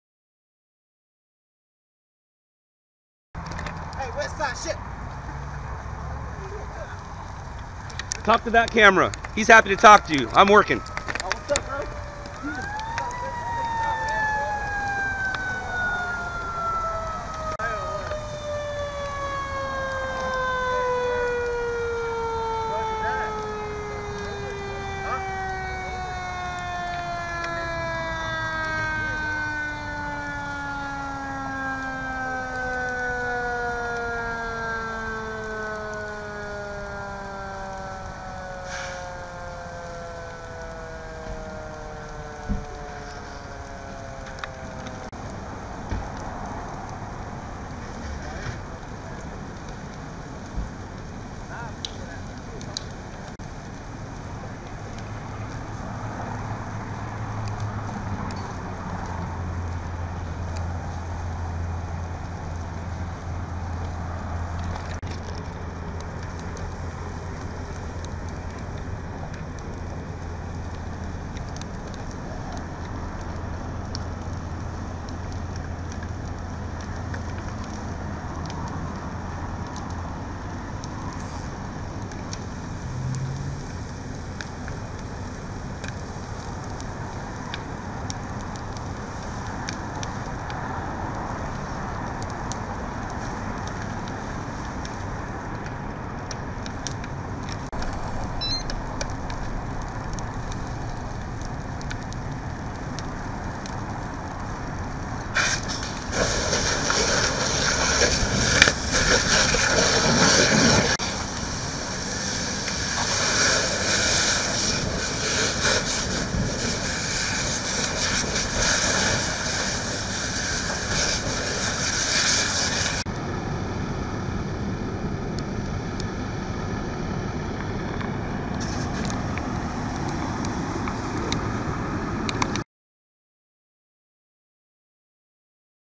Truck Fire / South LA RAW FOOTAGE
SOUTH LA - LAFD extinguished a truck fire that they said was a possible stolen vehicle. No injuries to report.